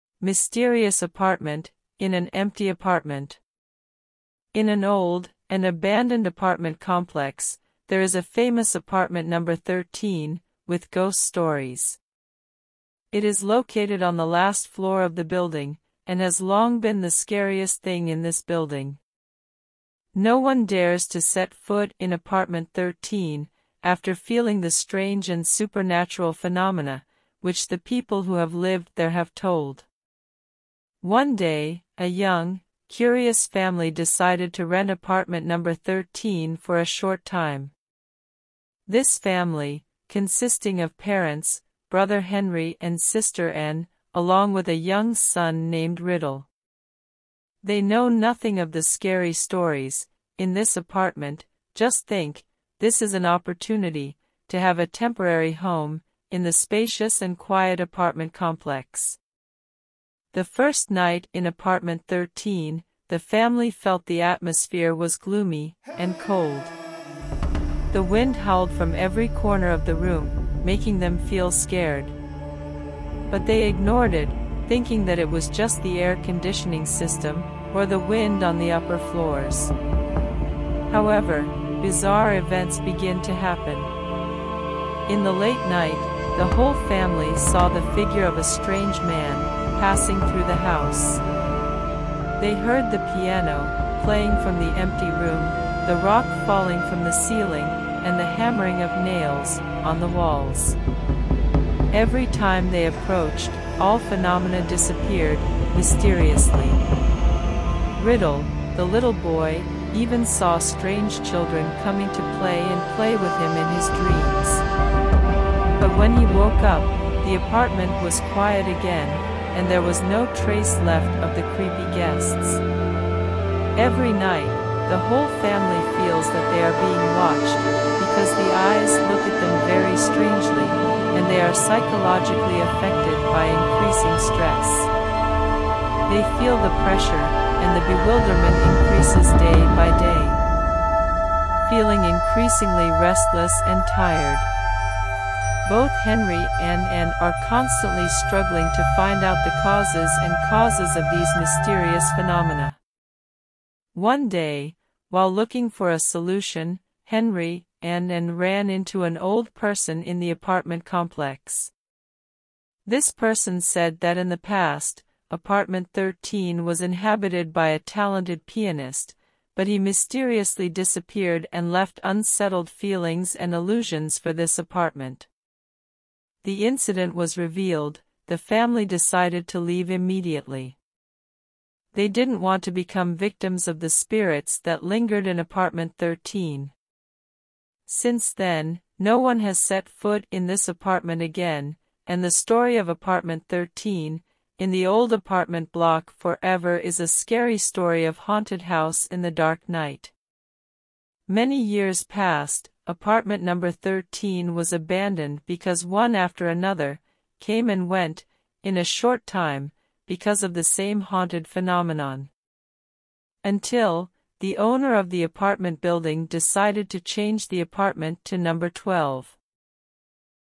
Mysterious apartment...
In an old, and abandoned apartment complex, there is a famous apartment number 13, with ghost stories. It is located on the last floor of the building, and has long been the scariest thing in this building. No one dares to set foot in apartment 13, after feeling the strange and supernatural phenomena, which the people who have lived there have told.